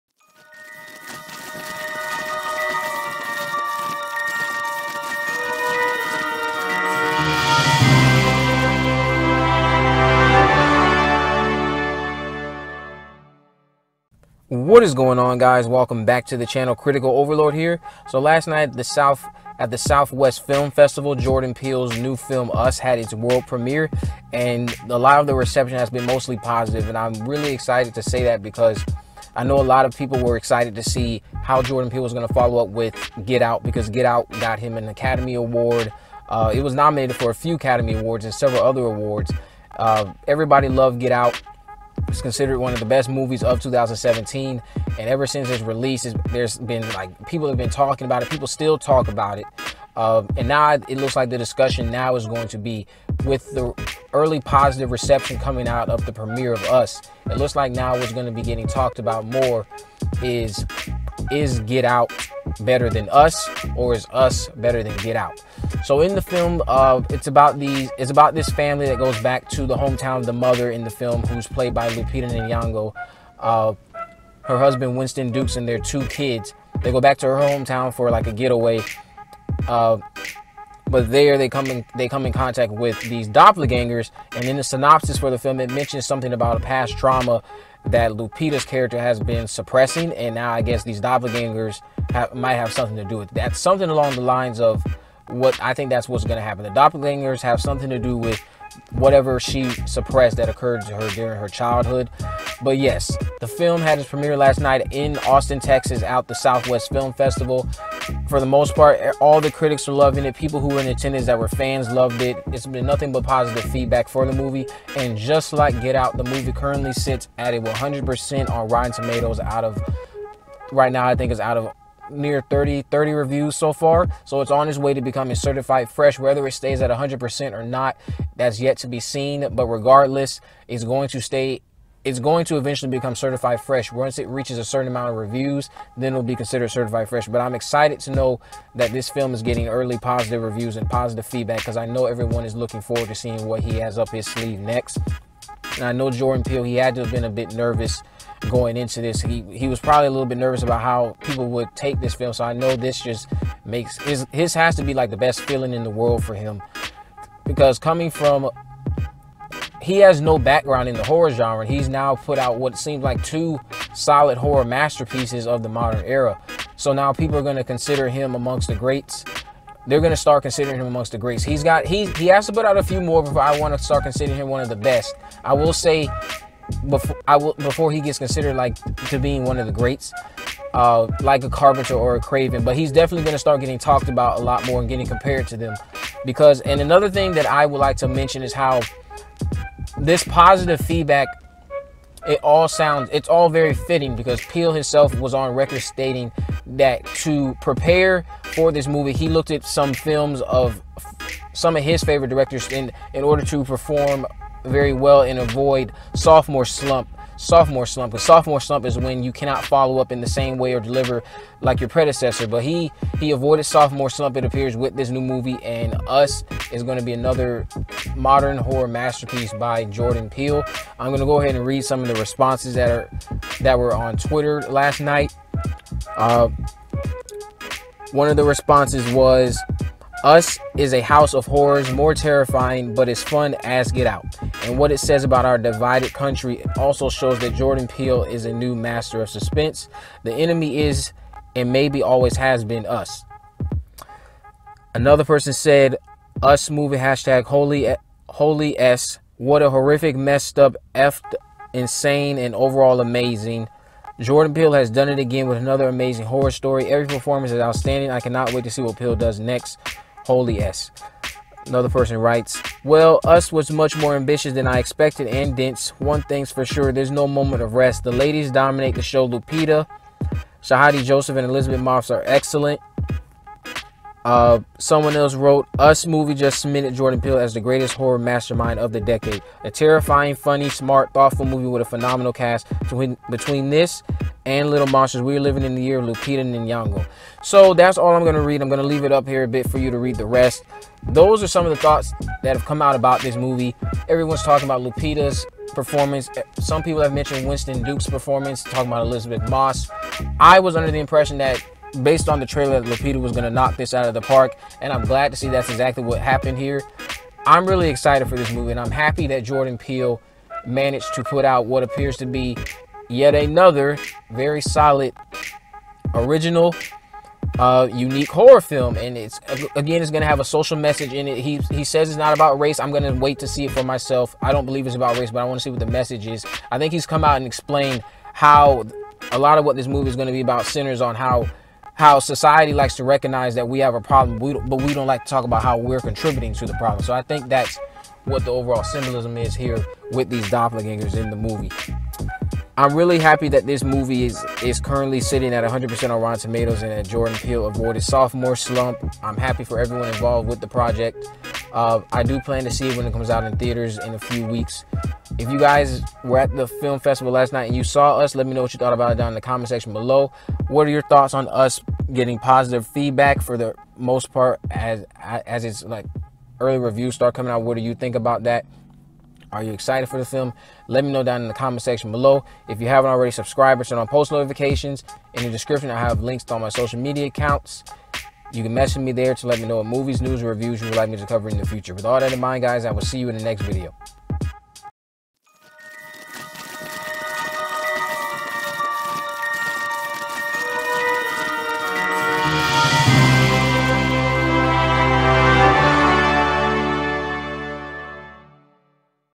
Us (2019) | Film Premieres in SXSW and Receives Positive Feedback
Jordan Peele's latest horror film Us premiered last night at the SXSW in Austin, Texas. Most of the feedback has been positive, which is good because many were worried Peele wouldn't live up to the success of Get Out.